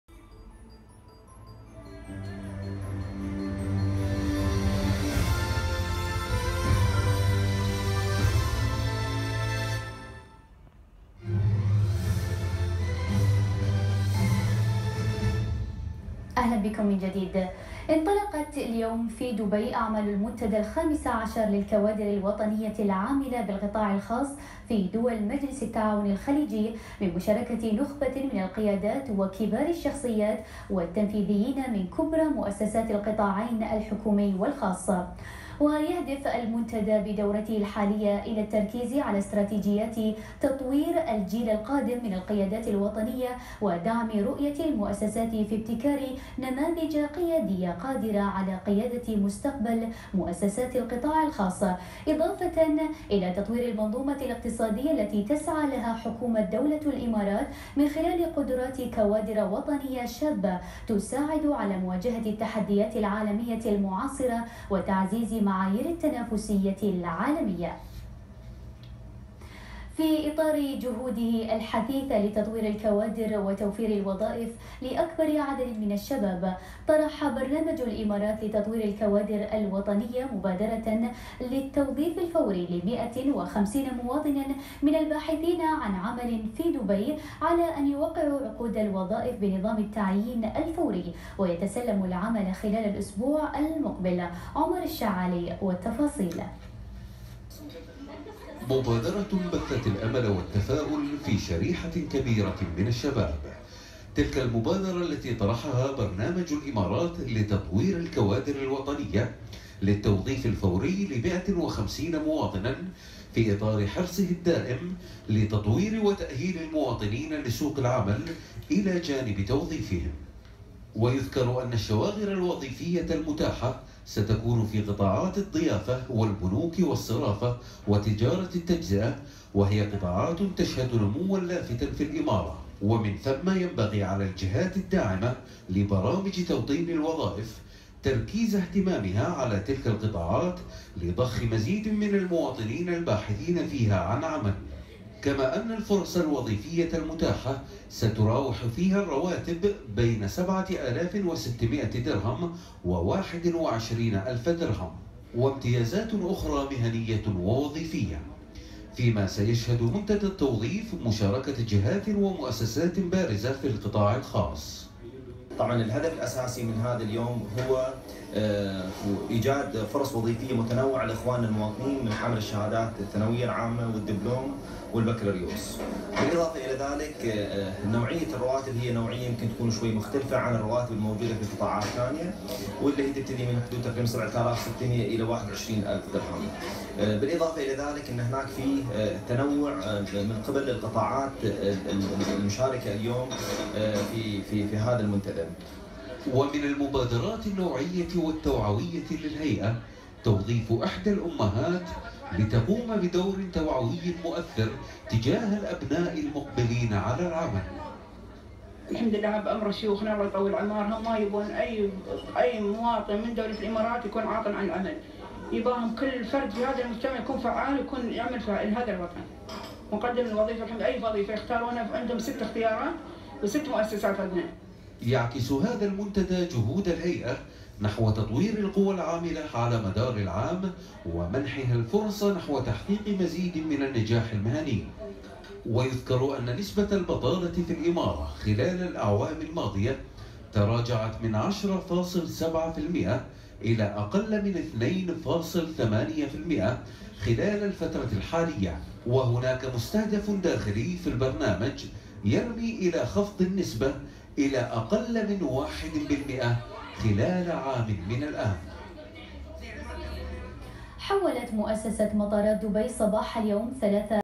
UAE Exchange participating to ENDP Forum to immediately Hire 150 UAE local staff
كوادر»: توظيف فوري لـ 150 مواطناً غداً
التاريخ:: 27 ديسمبر 2016
المصدر:
• محمد عثمان ـــ دبي

أفاد رئيس الكوادر الوطنية في برنامج الإمارات لتطوير الكوادر الوطنية (كوادر)، عيسى الملا، بأن «البرنامج سيطرح غداً، مبادرة للتوظيف الفوري لـ150 مواطناً من الباحثين عن عمل في الإمارة، على أن يوقّعوا عقود الوظائف بنظام التعيين الفوري، ويتسلموا العمل خلال الأسبوع المقبل».
وقال الملا لـ«الإمارات اليوم»، إن «الشواغر الوظيفية المتاحة ستكون في قطاعات الضيافة، والبنوك والصرافة، وتجارة التجزئة، وهي قطاعات تشهد نمواً لافتاً في الإمارة، ومن ثم ينبغي على الجهات الداعمة لبرامج توطين الوظائف تركيز اهتمامها على تلك القطاعات لضخّ مزيد من المواطنين الباحثين عن عمل فيها».
وأضاف أن «الوظائف المزمع طرحها ضمن منتدى متخصص للتوظيف سيعقد في فندق العنوان في دبي، ويضم وظائف في خدمة العملاء، والمبيعات، ووظائف إشرافية، فضلاً عن برامج متخصصة للخريجين الجدد من حملة الشهادات العلمية المختلفة، كالثانوية العامة، والدبلومات الفنية، والبكالوريوس».
وذكر أن «الفرص الوظيفية المتاحة ستراوح فيها الرواتب بينن 7600 درهم و21 ألف درهم، وامتيازات أخرى مهنية ووظيفية، فيما سيشهد منتدى التوظيف مشاركة جهات ومؤسسات بارزة في القطاع الخاص، منها مجموعة إلياس ومصطفى كلداري، وجمعية الاتحاد التعاونية، وبنك المشرق، وبنك دبي الإسلامي، ومجموعة شركات شلهوب، وشركة الإمارات للصرافة».
ولفت الملا، إلى أنه «تم العمل على الإعداد لمنتدى التوظيف ليكون ختاماً إيجابياً للعام الجاري، وسندخل به العام الجديد، من خلال تنويع الفرص المتاحة لمواطنين في قطاعات عمل مختلفة، وذلك لإعطاء أكبر قدر ممكن من المرونة للباحثين عن عمل».
وحسب الملا، فإن «عدد الشركات في دبي، التي تنوي توظيف كفاءات مواطنة، زاد خلال العام الجاري مقارنة بالأعوام الماضية»، ويرجع ذلك إلى «حرص الشركات على توفير أكبر قدر ممكن من الوظائف، خصوصاً في نهاية العام، حتى تكمل فرق عملها الإدارية والإشرافية مع دخول العام الجديد».
واعتبر أن «(منتدى التوظيف) يعكس جهود الهيئة نحو تطوير القوى العاملة على مدار العام، ومنحها الفرص نحو تحقيق مزيد من النجاح المهني»، معرباً عن «تقديره لشركاء برنامج الإمارات لتطوير الكوادر الوطنية، من شركات ومؤسسات القطاع الخاص، التي أسهمت ـ ومازالت ـ في عملية التوظيف، خصوصاً المشاركة في منتدى التوظيف».
وحسب بيانات برنامج الإمارات لتطوير الكوادر الوطنية، التابع لهيئة المعرفة والتنمية البشرية، فإنه خلال الفترة الماضية نجح البرنامج في تحقيق نتائج بارزة، أهمها المساهمة في توظيف ما يناهز الـ13 ألف مواطن في مؤسسات وشركات عاملة في القطاع الخاص.
وقال الملا إن «نسبة البطالة المسجلة في الإمارة خلال الأعوام الماضية، تراجعت من 10.7% إلى أقل من 2.8% خلال الفترة الحالية، وهناك مستهدف داخلي في البرنامج يرمي إلى خفض النسبة إلى أقل من 1% بحلول عام 2021».
دعم حكومي
نوّه رئيس الكوادر الوطنية في برنامج الإمارات لتطوير الكوادر الوطنية (كوادر)، عيسى الملا، بحرص جهات حكومية وخاصة على تأهيل المواطنين لسوق العمل في الإمارة، وأبرزها المجلس التنفيذي لحكومة دبي، الذي يدعم دائماً مبادرات تأهيل الكوادر المواطنة في الإمارة، وكان آخر جهود التنسيق المشتركة في هذا الشأن، البدء في وضع تصوّر سنوي للبطالة في دبي، واعتماده معياراً رسمياً في الإمارة.
وتابع: «كانت طروحات المجلس التنفيذي في دبي تسبق فكرة مجرد توفير وظائف للمواطنين، إلى وضع مؤشر ثابت لقياس ما تحقق من منجزات في هذا الصدد، وهو يعكس حرص المجلس على توفير بيانات وأرقام واقعية حول وضعية المواطنين الباحثين عن عمل في الإمارة، فيما تشترك مع البرنامج مؤسسات تعليمية وأكاديمية حكومية وخاصة، في تطوير الفكرة وتطبيقها على أرض الواقع».